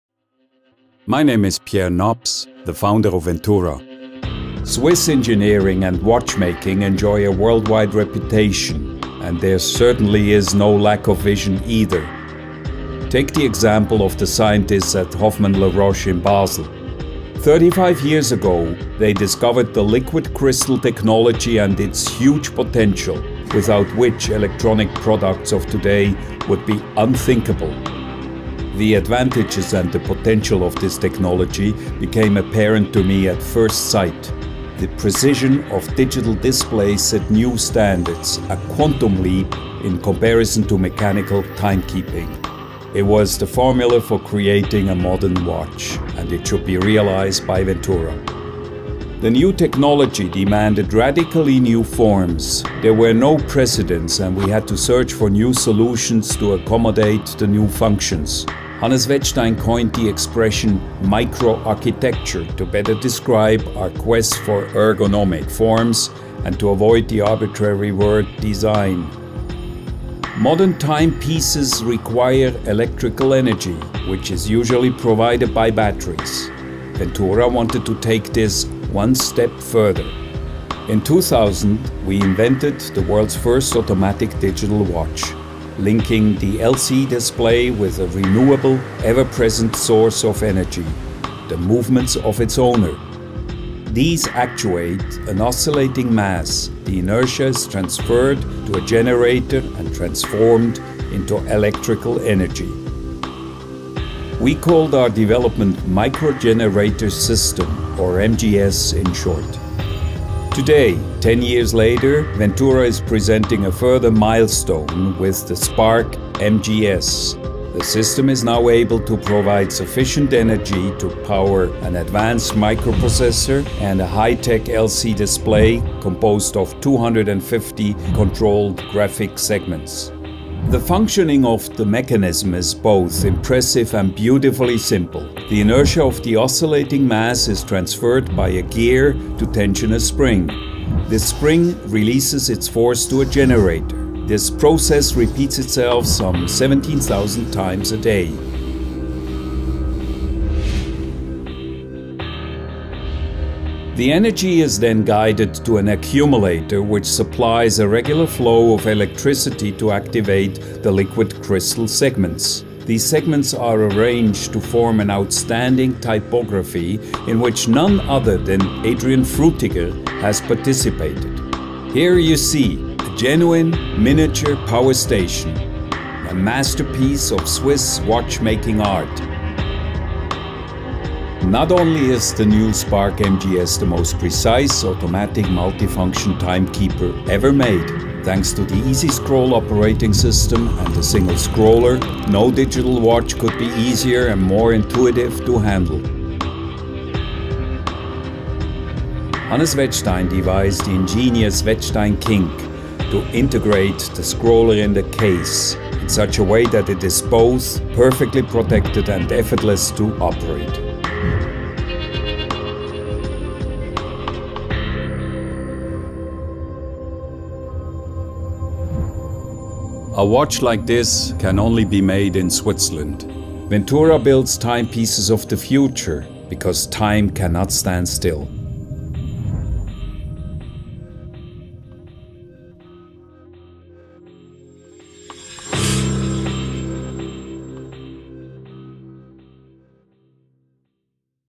VENTURA SPARC MGS
Ventura invented the Automatic Digital Watch in 2000. The movements of the wrist activate a rotary mass which relays the momentum to a micro-generator; the electric energy gained in the process continuously feeds an optoelectronic time-module. The Micro-Generator-System of the new SPARC MGS was developed to achieve maximum performance. Placing the MGS side-by-side next to the time-device enabled a larger and heavier oscillating mass. This array also permits the viewing of the mechanism from the top, giving the SPARC MGS its characteristic appearance. The miniature power-station sustains a state-of-the-art micro-processor and a 250-segments 12-digit liquid-crystal display. The latest operating system EasySkroll v.2.0 allows later upgrades, another first in wristwatches. The multiple functions of the SPARC MGS are intuitively operated by a single scroller. Ventura innovates.